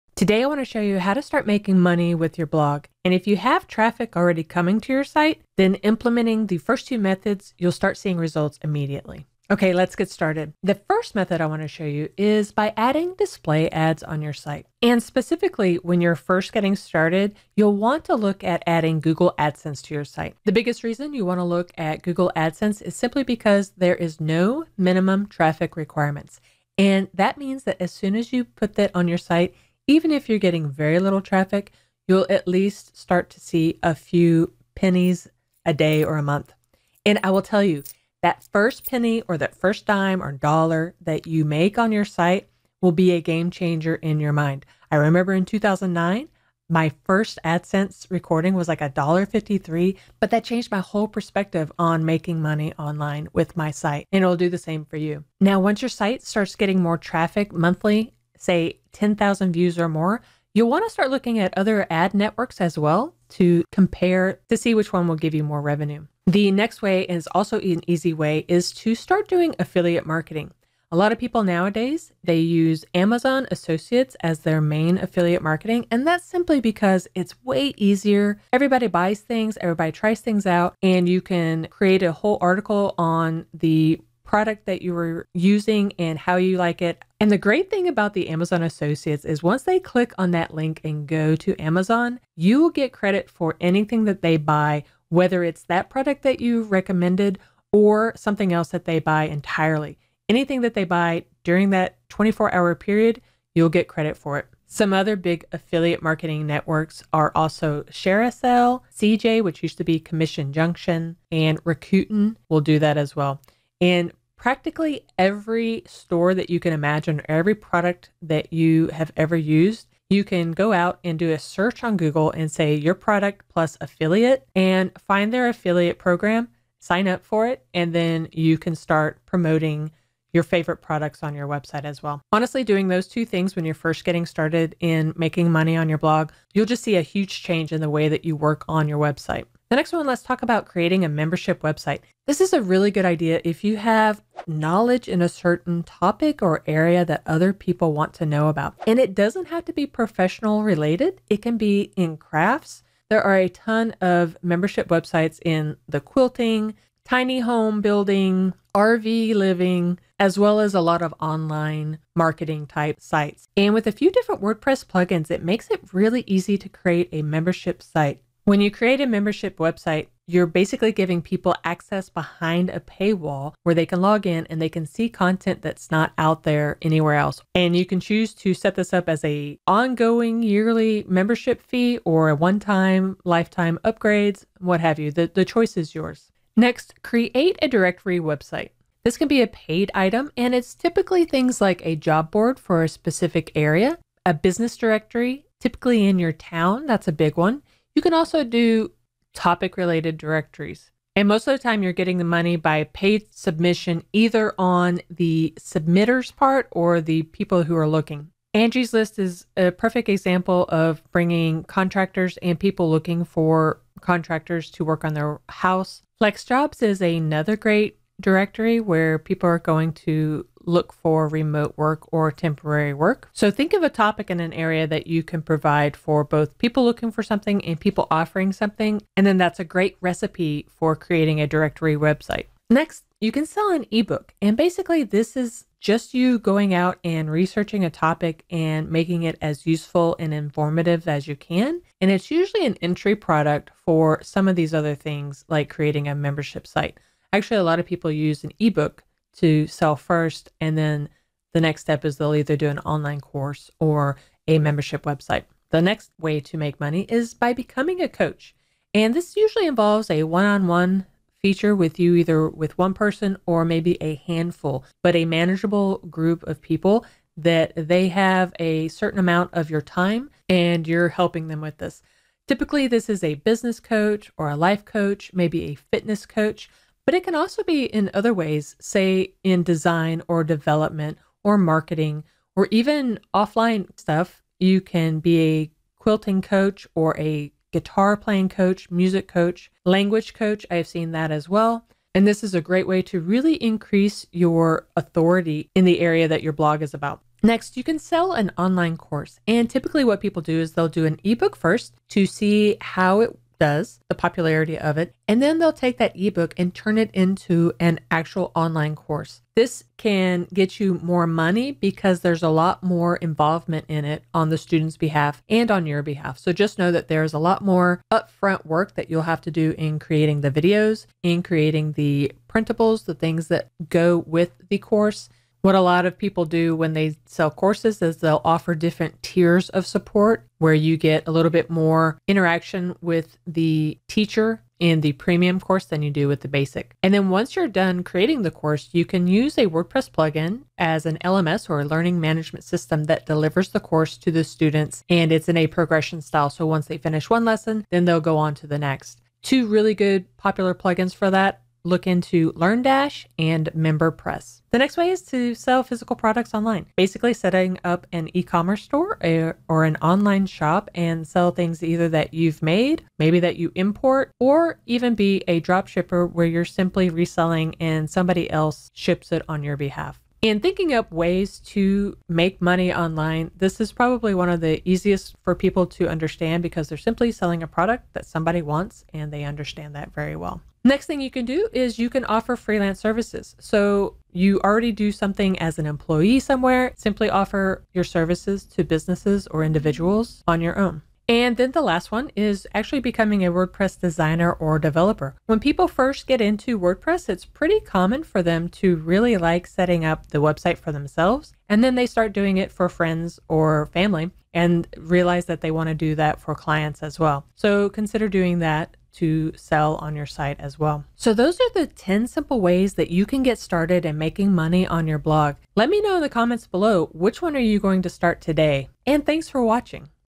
10 Simple Ways to Make Money with Your Blog [Today]
With the blog set up, most users look for ways to monetize the blog to at least pay for the site and keep it running. There are multiple different ways but in this video, we will show you 10 simple methods you can use to start making money with your WordPress blog.

Display ads - For this, one of the best starting points would be to take a look into Google Adsense or other ad providers as your site grows in popularity.

Affiliate Marketing - Take a look at some of your favorite products that you would write about for your site and see if they have an affiliate program to pay you for promoting their product or service.

Membership website - Creating a membership website allows you to share your content to your audience behind a paywall and add other incentivizing features for your visitors.

Sell an ebook - Take the information you know in a specific subject and gather it together in an ebook to sell to users to help expand their knowledge in that subject.

Become a Coach - There are likely people who want one on one training with a specific subject that you're knowledgeable about and you can sell one on one training through your website to help them learn.

Sell online courses - This is similar to a membership site but would require more community interaction for answering questions that your community asks about your tutorials.

Sell Products - Set up ecommerce tools on your sites such as WooCommerce or a dropshipping solution so you can sell your products directly to your visitors or look into dropshipping.

Offer freelance services - Offer your services that you use in one profession or similar and have users pay for you to do this service for them.

Work on WordPress - You've set up your site on WordPress including articles, a store or similar. You can sell your knowledge to help someone get their WordPress site up and running.